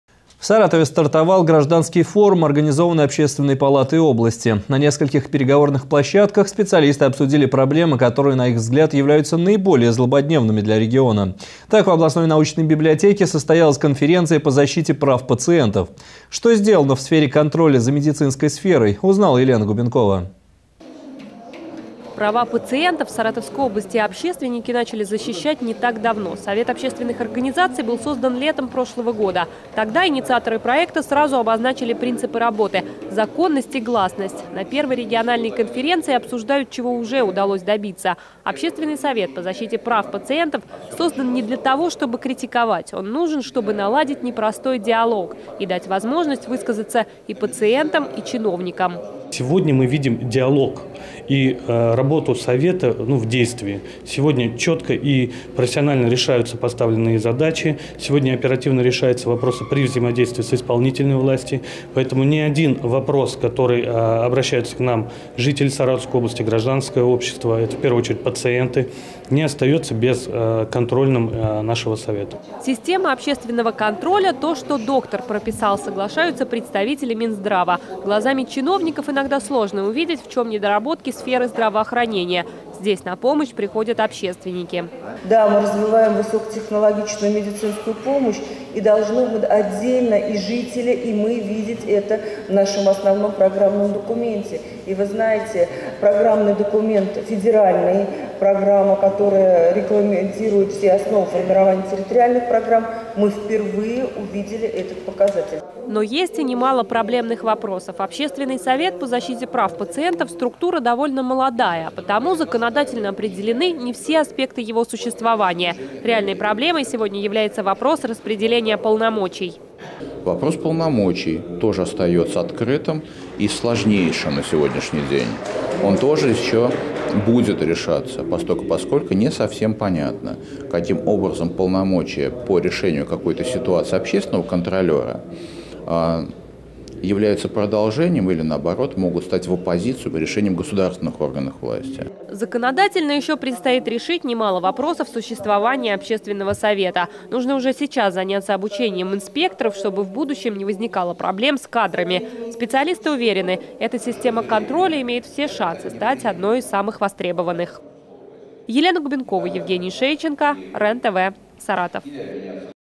На Гражданском форуме специалисты обсудили проблемы региона
В Саратове стартовал гражданский форум, организованный общественной палатой области. На нескольких переговорных площадках специалисты обсудили проблемы, которые, на их взгляд, являются наиболее злободневными для региона. Что сделано в сфере контроля за медицинской сферой, узнала Елена Губенкова. Сюжет вышел в эфир 9 декабря.